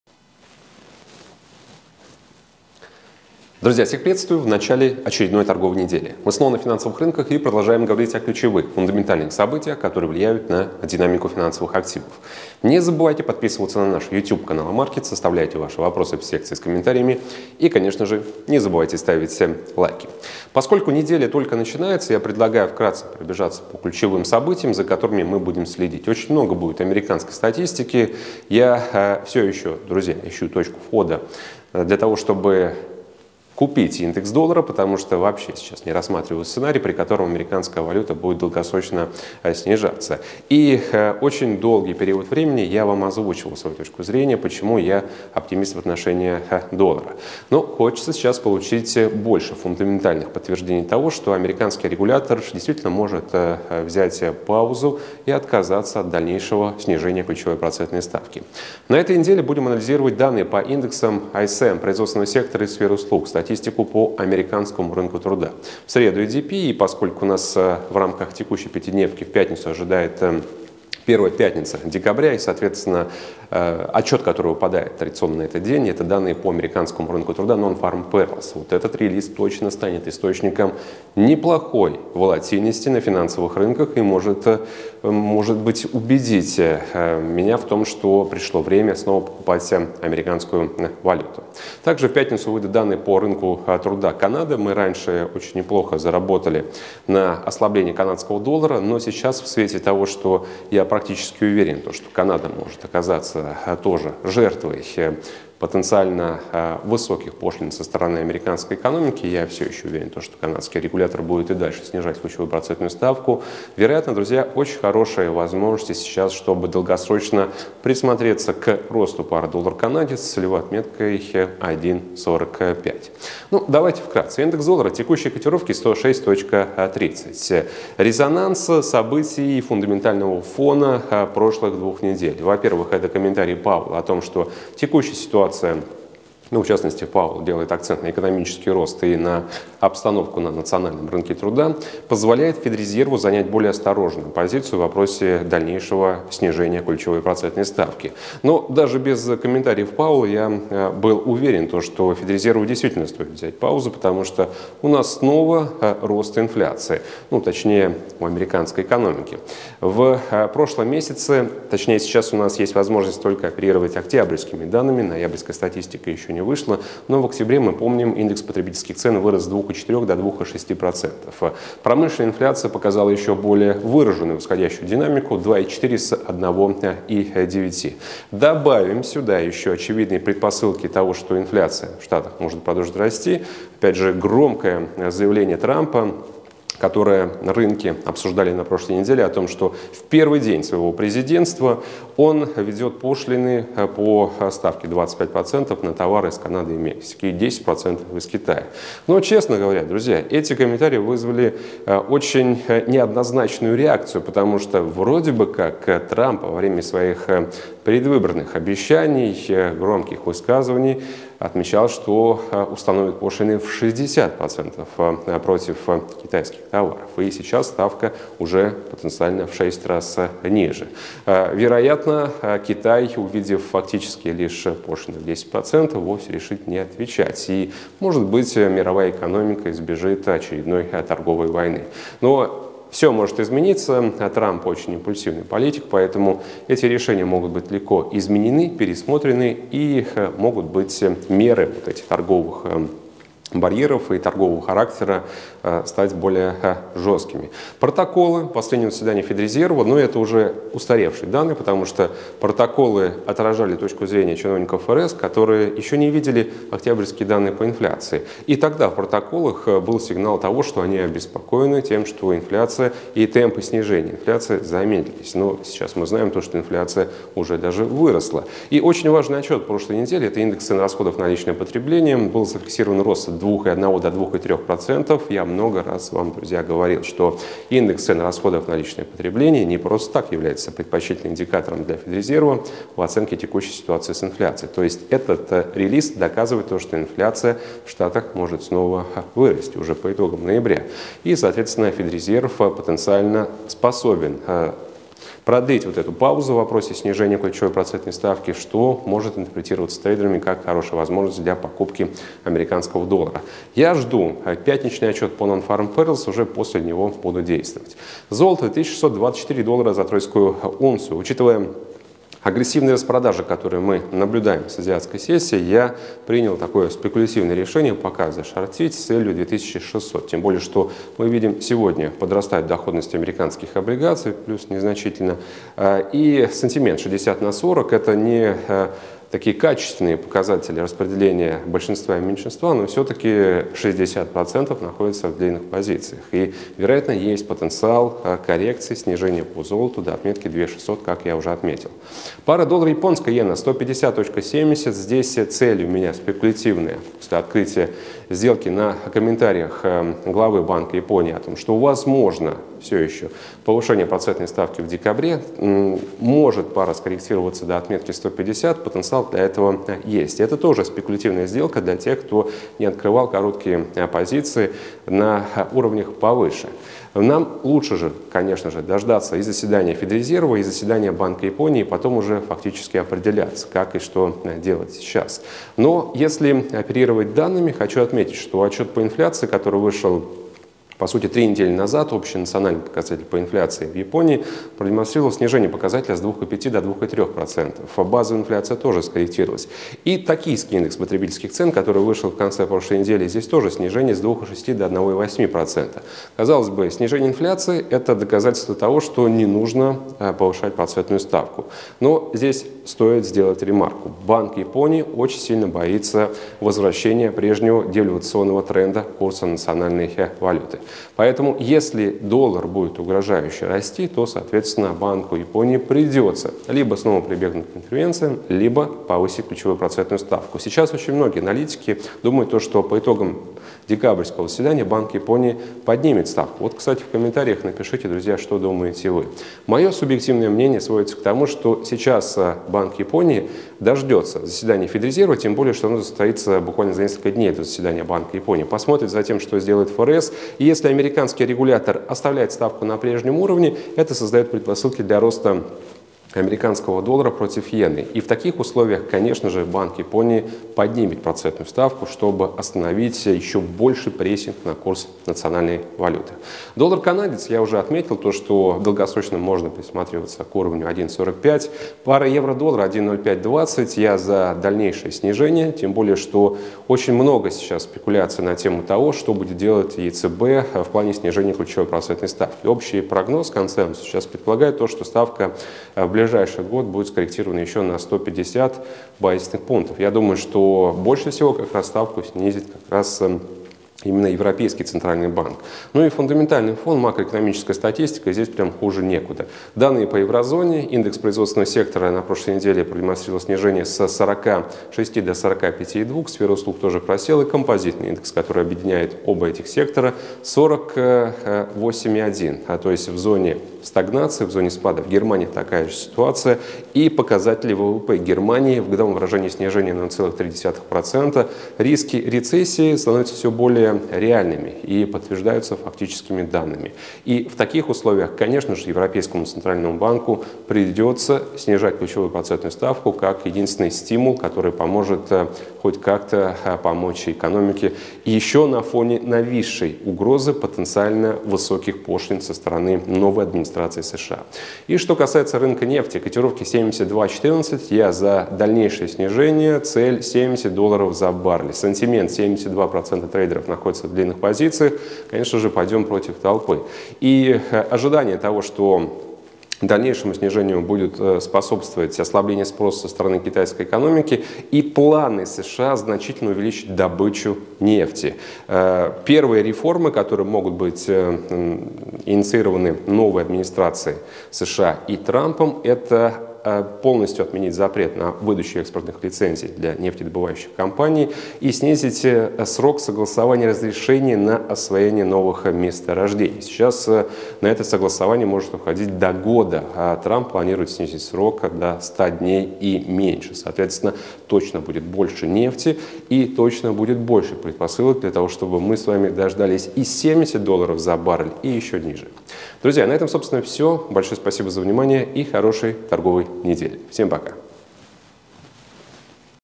Доллар: чего ждать в декабре? | AMarkets
Чего ждут от Трампа, что такое Trump trade, о выборах в США, их последствия для финансовых рынков, о заседании ФРС, о данных nonfarm payrolls, золото перешло к коррекции, как Трамп влияет на доллар и золото, о данных по инфляции в США, что поддержало позиции доллара, о перспективах доллара, что решила ФРС, что сказал Пауэлл на пресс-конференции, о фунте и потенциале роста евро, о рынке труда США,  как заработать на золоте, от чего зависит решение ФРС по ключевой процентной ставке